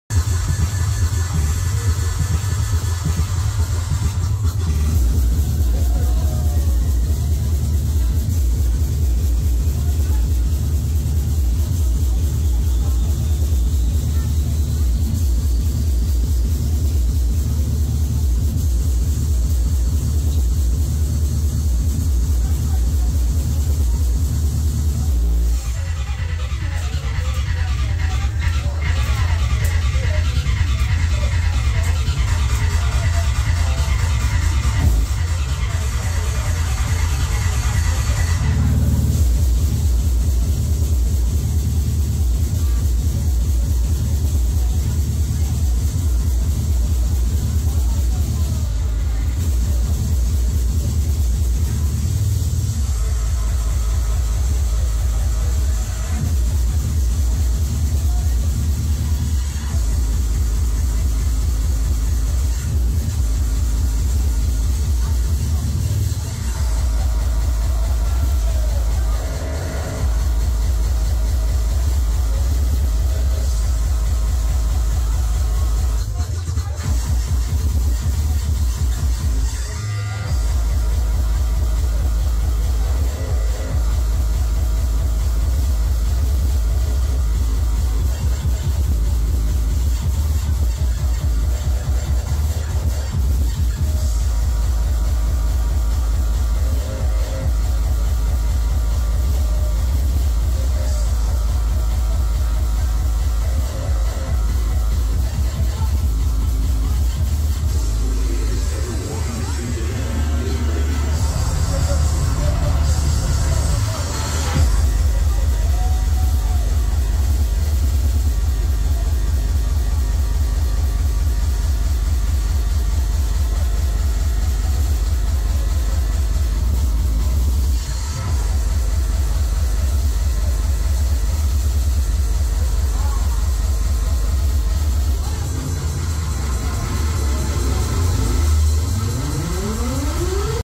Harder Christmas 2022 (3/3), Stage Winter Wonderland: Tripped.
Early Terror set by DJ Tripped at “Harder Christmas 2022” in Club Vios.

Artist/DJ: Tripped.

Location: Club Vios, Hengelo, Netherlands.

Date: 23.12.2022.

Shot with: Samsung Galaxy S22 Ultra.